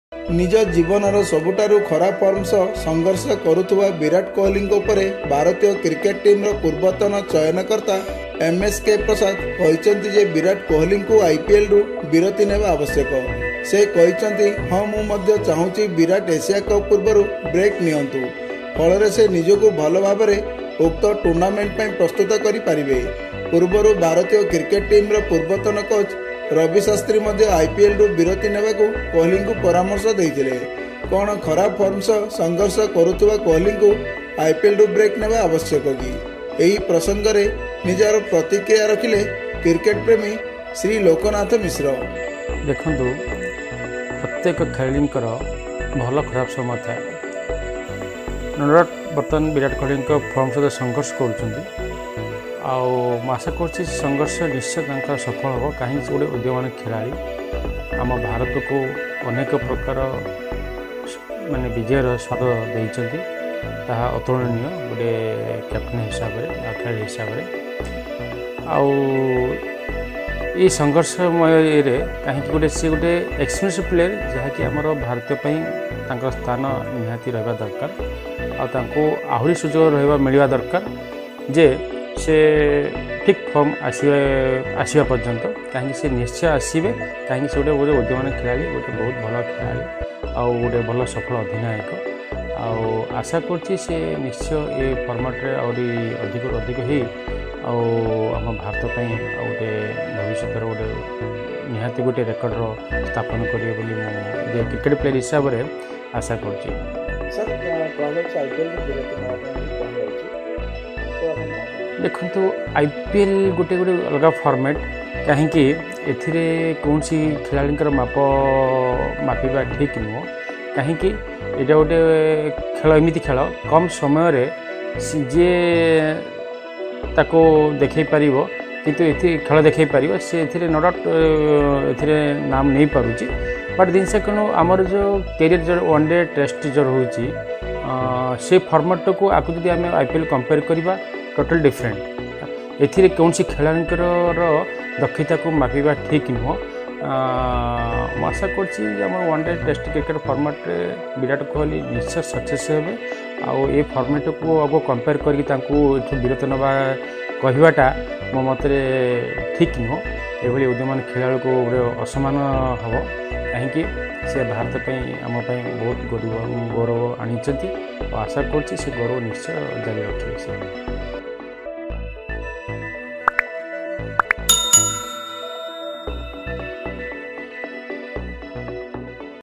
ଆଇପିଏଲରୁ ବିରତି ନେବା ଉଚିତ ବିରାଟ କୋହଲି: ଭାରତୀୟ ଟିମ୍‌ର ପୂର୍ବତନ ଚୟନକର୍ତ୍ତା ପ୍ରସାଦ
ak14tv.in - 9/5/2022